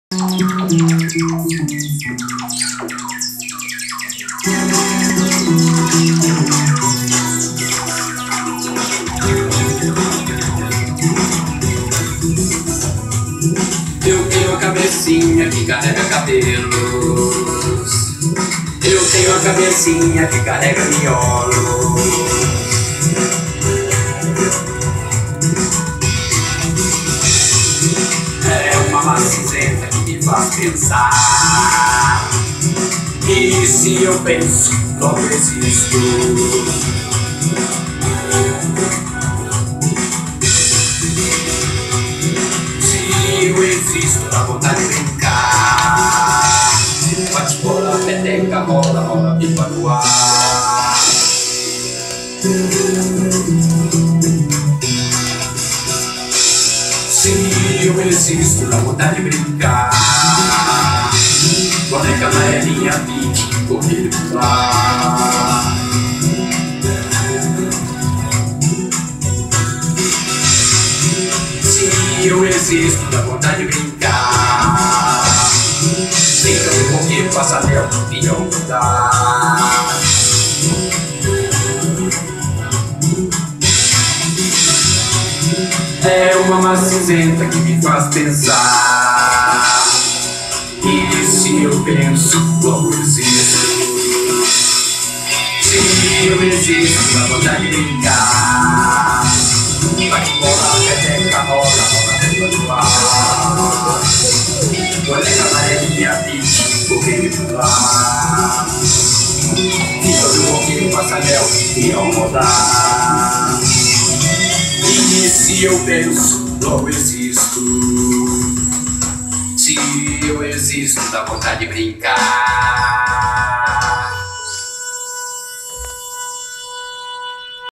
1 VONTADE BRINCAR
O palhaço Doctor Dinno cantando e equilibrando no monociclo faz malabares e com cores vivas Gabriela, Salomé e Lua Hara, três performáticas atrizes coreógrafa os dizeres que ele canta.

Contribuições: BB conta poupança 62632-5 agência 3895-4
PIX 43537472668